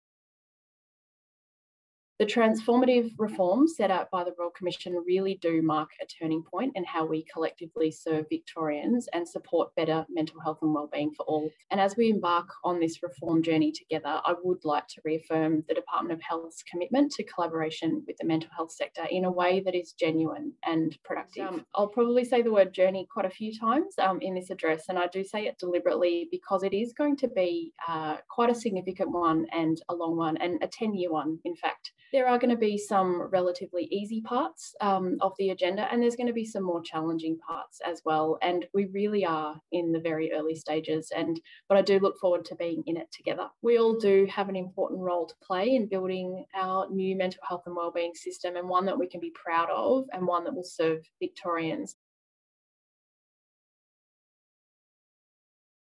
Preparing for Reform with Katherine Whetton
Katherine Whetton, Deputy Secretary of Mental Health at the Victorian Department of Health & Human Services, addresses a symposium on preparing for reform in the state's mental health sector.

Preparing for reform: Co-production, culture change and leadership
29 June 2021

Part of the Mental Health and Suicide Prevention Symposium Series hosted by Mental Health Victoria